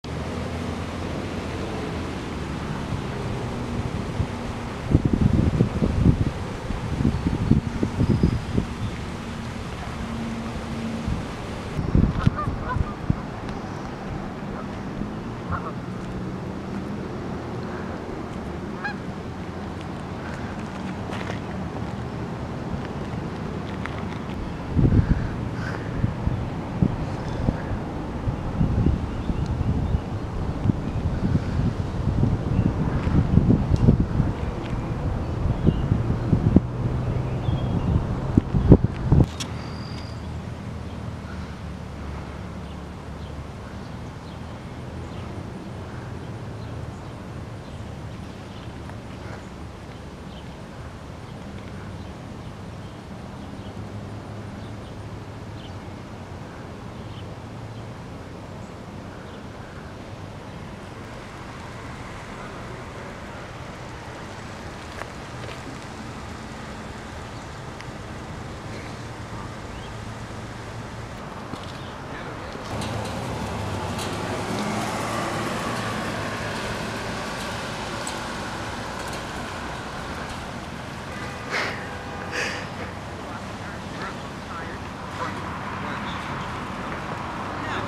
Deerhead Lake Geese
Deerhead Lake in Forked River has a geese problem.